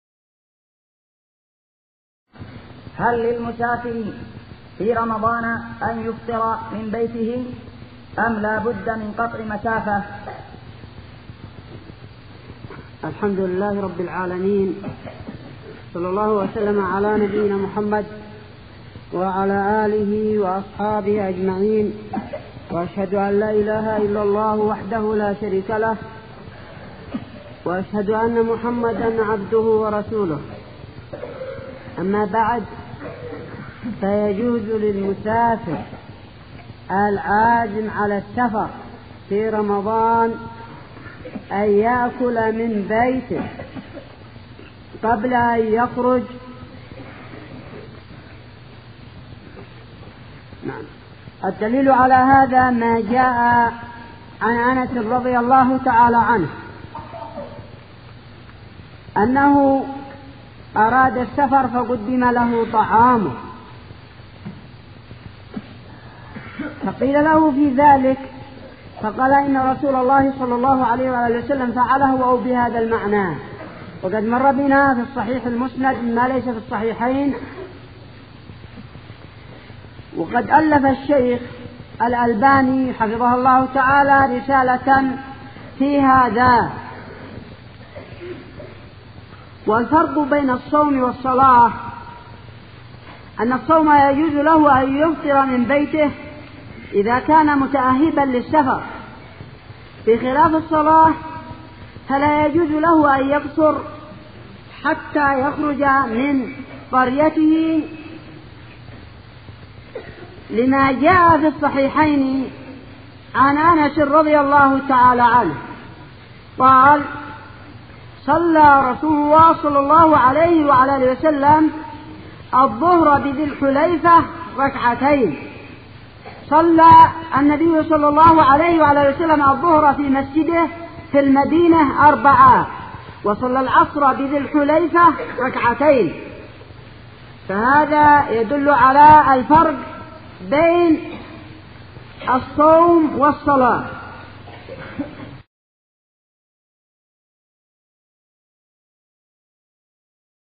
le voyageur
L'avis le plus fort est que le voyageur -quand il est déterminé de voyager- a le droit de boire et manger à domicile avant de quitter sa résidence.
La preuve à cela: le hadith d'Anas -qu'Allah l'agrée.
Shaykh Al-Albani a composé une épître dans ce sujet.
Le jeûneur diffère en cela du prieur qui n'utilise les permissions relatives à la prière que lorsqu'il quitte sa résidence.

Shaykh Moqbil Al-Wadi'i qu'Allah lui fasse miséricorde.